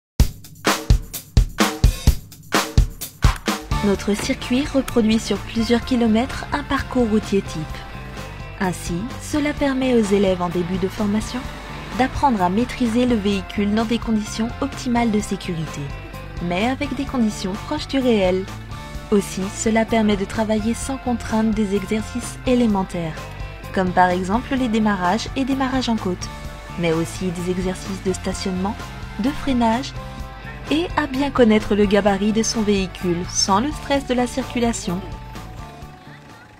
Découverte en image de quelques points clés du circuit de formation
Parcours routier type pour cette auto-école, qui propose de nombreux exercices d'entraînement dans des conditions proches de la réalité, mais en toute sécurité. Film corporate externe réalisé sur le circuit ayant un objectif cognitif.